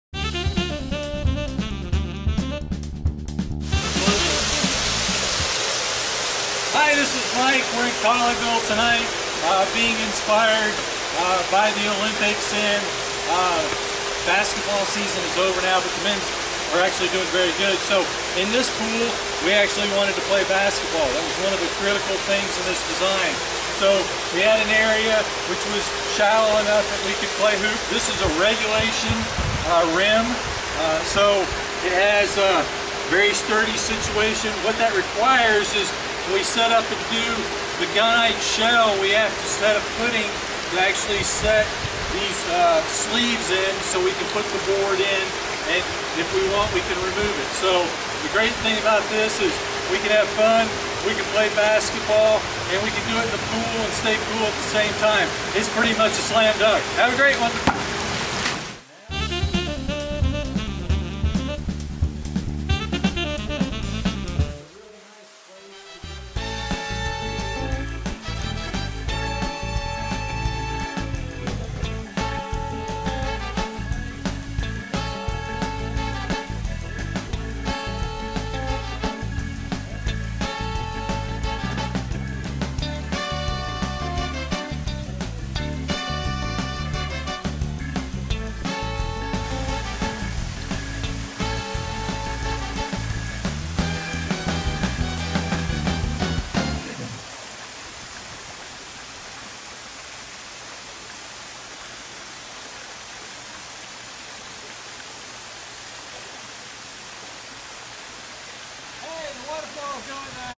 What Is It -Built In Basketball Goal
Texas swimming pool designer Mike Farley gives insights about installing a built in basketball Goal while building an in ground swimming pool or Lazy River.  Swimming pool (Lazy River) designed by Mike Farley and constructed by Claffey Pools in Colleyville, TX.  This project was featured on HGTV's Cool Pools.